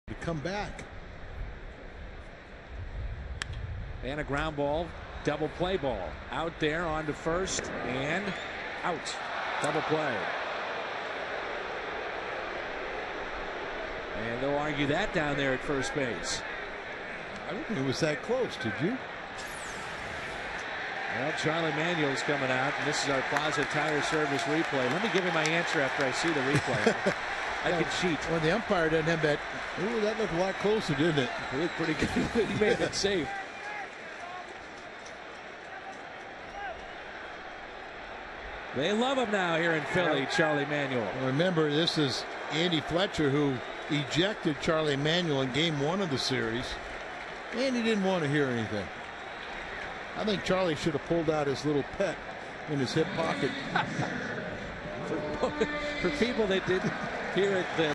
2010/05/06 Freese turns two
STL@PHI: Freese starts a double play to end the fifth